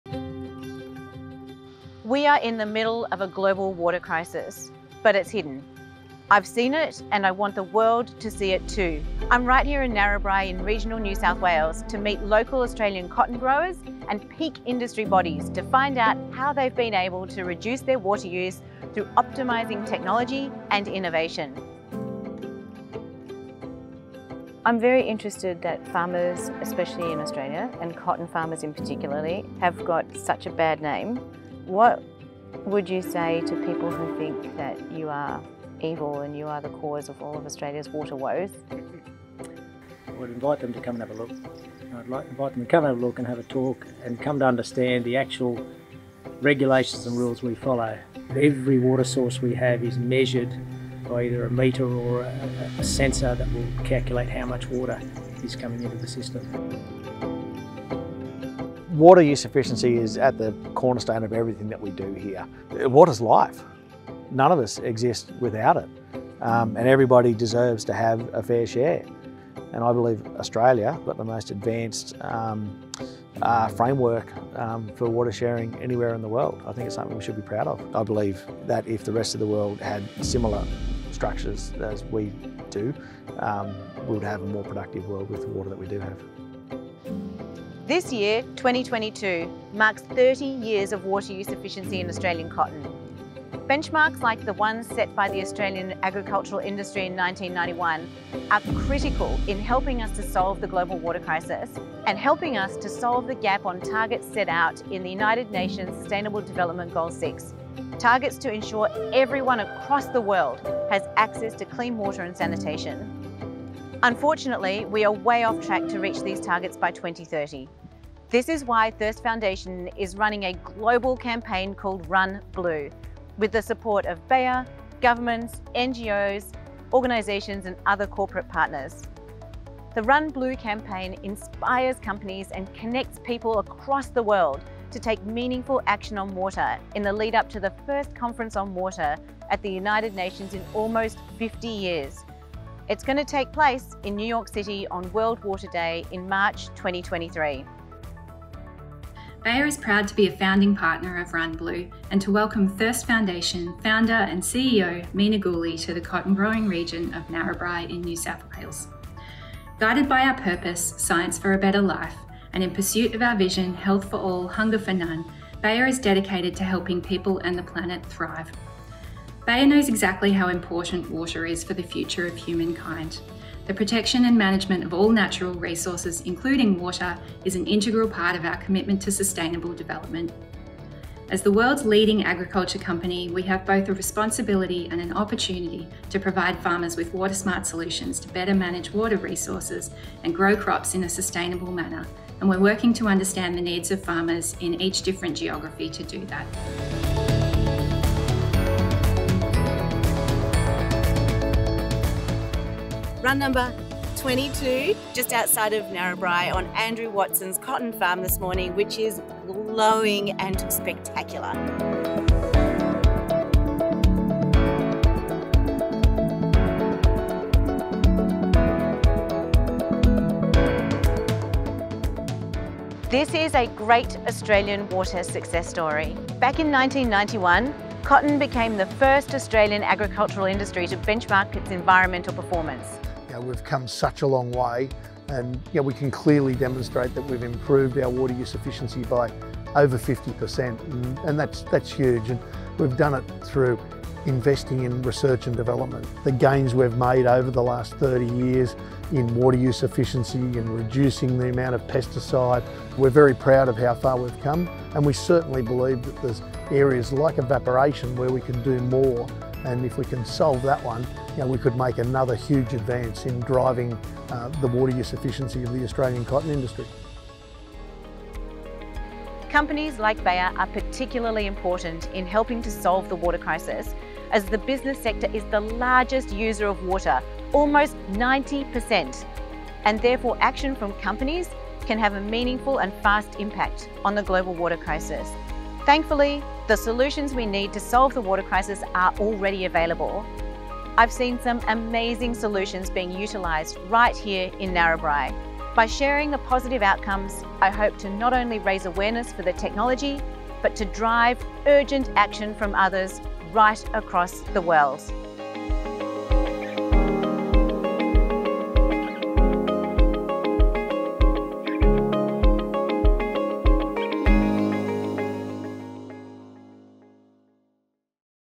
Mina Guli meets with Australian cotton farmers and Bayer
During her RunBlue campaign, Mina Guli took some time out to visit cotton farmers in Australia to find out more about their water usage and how they maximise efficiency while farming their crops.
She also had a round table discussion with the Bayer team to chat about how water can be best managed as part of their commitment to sustainable development.